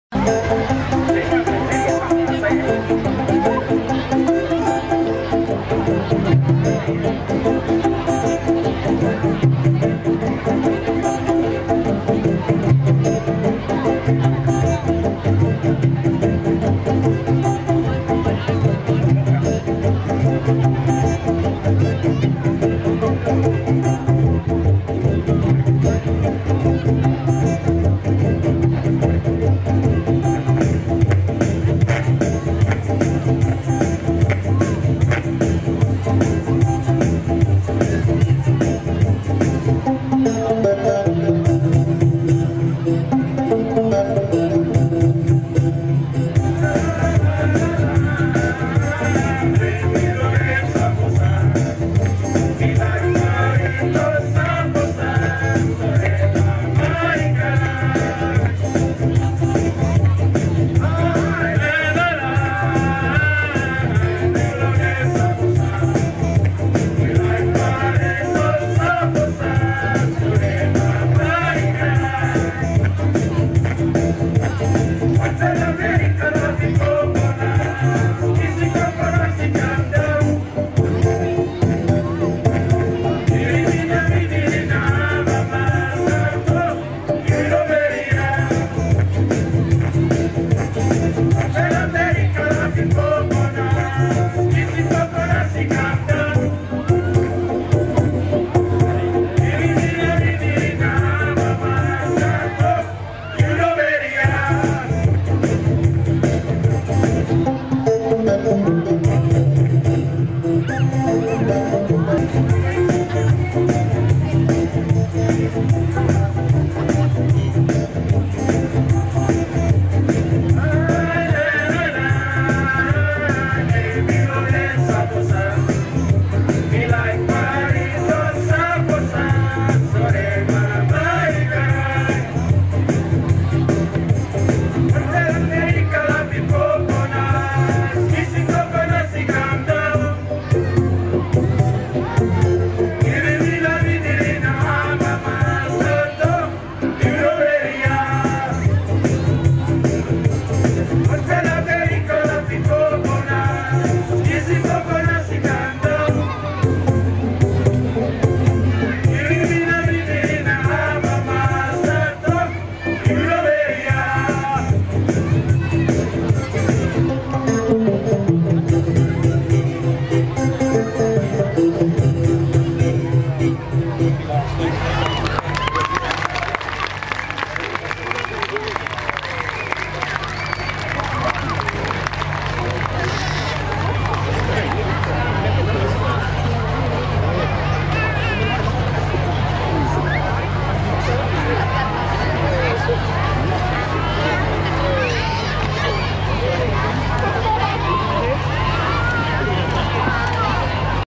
Saposa Dance @ Emerald CHMS 2013
Dance and song from North Solomon Province of PNG performed in Emerald Central Highlands Multicultural Show (CHMS), Qld Australia on the 24th of August 2013.